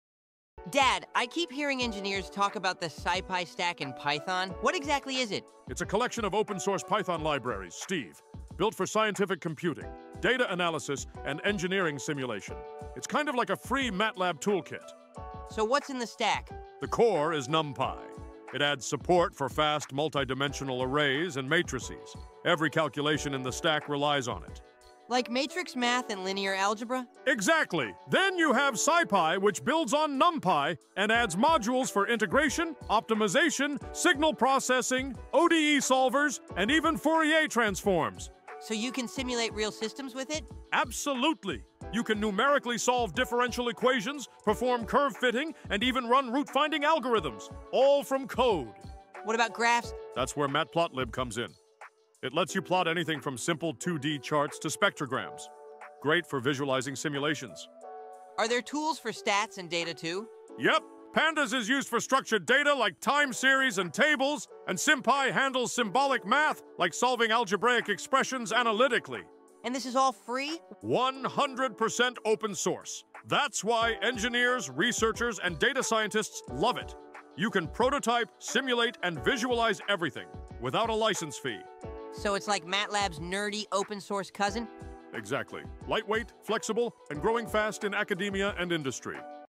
Why do engineers use Python's SciPy Stack?
From matrix math to simulations and data visualization, the SciPy stack gives engineers powerful tools, all in Python. Stan breaks down what it is and why it matters.